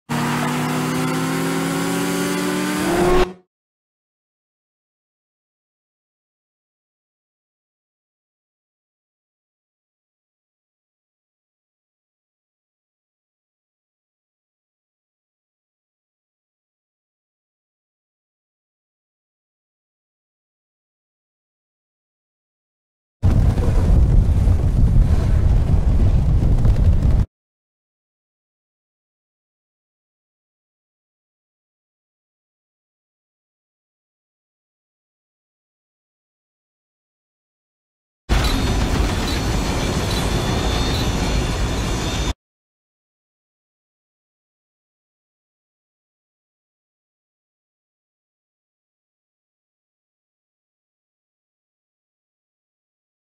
GMC Hummer EV truck teased with 1,000 horsepower and 0-60 mph in 3 seconds
The GMC Hummer EV will be revealed on May 20, 2020 and will be built in Michigan at General Motors’ Detroit-Hamtramck assembly plant. Available fall 2021.